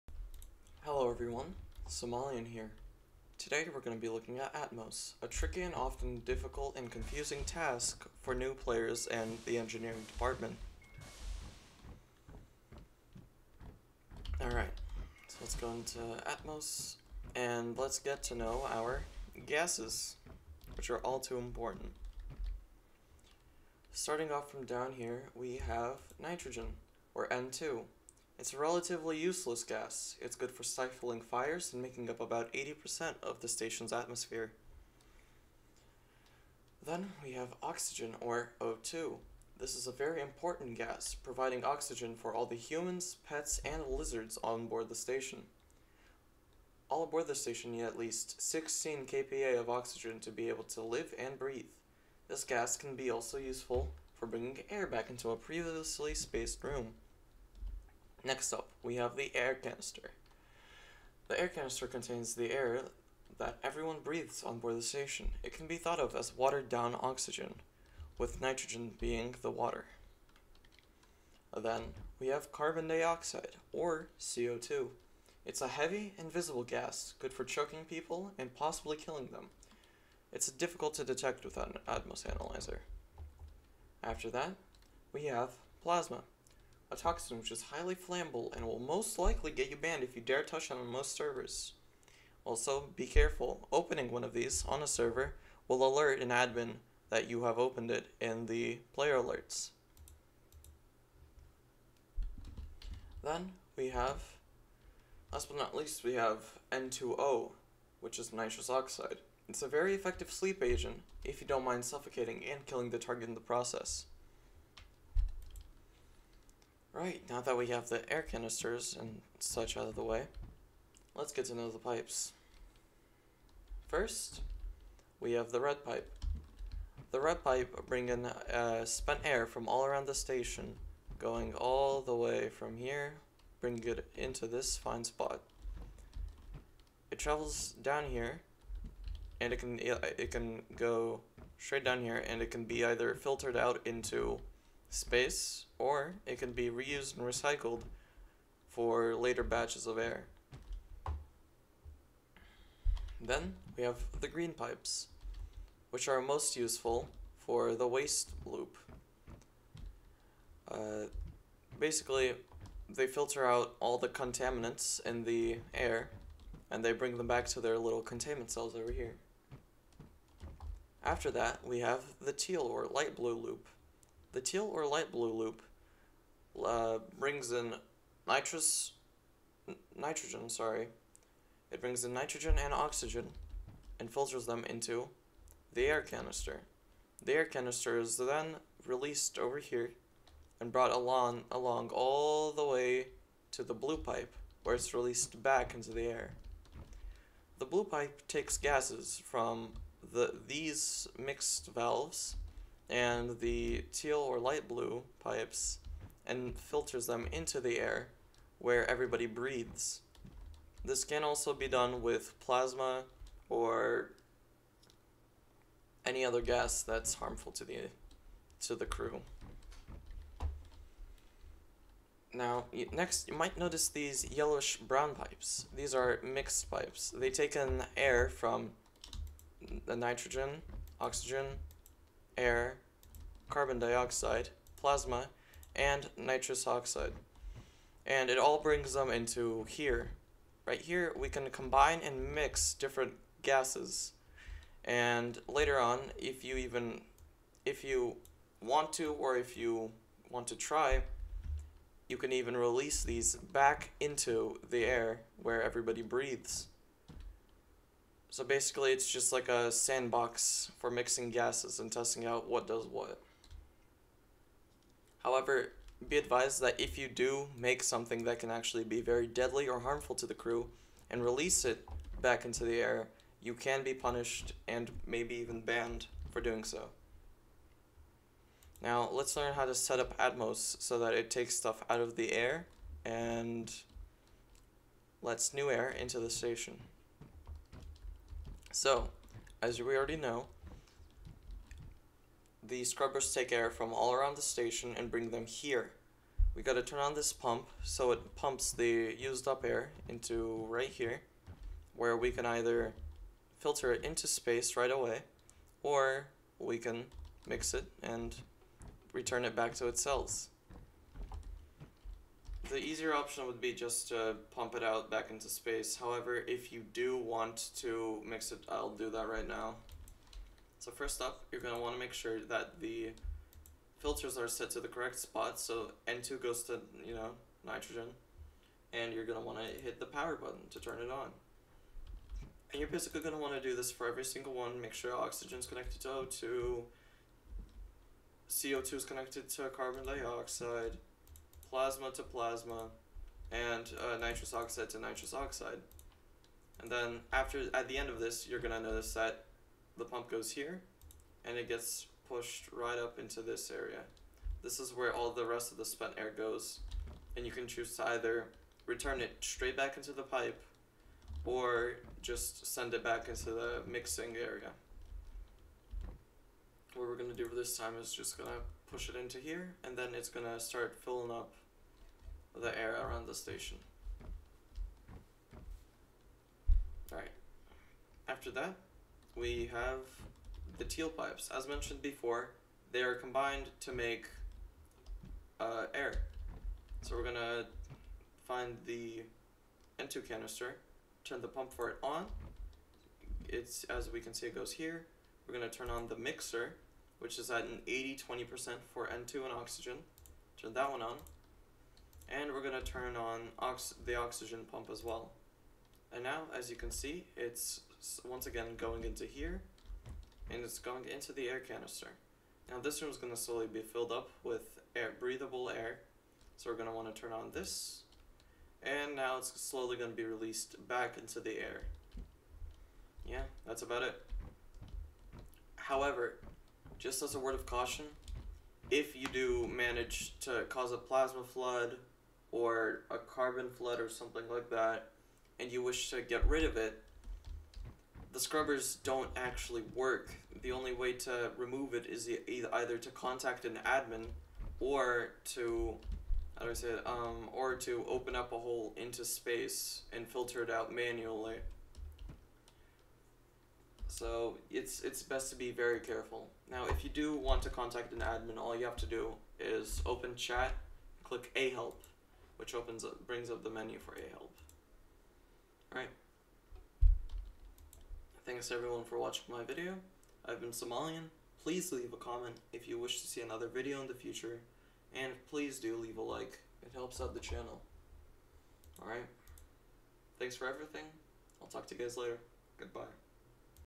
Atmos Tutorial
Today we’re going to be looking at atmos, a tricky and often confusing task for new players and the engineering department.
0:00 Intro
0:23 Gases
1:55 Pipes and Loops
4:37 Atmos Setup
7:05 Word of Caution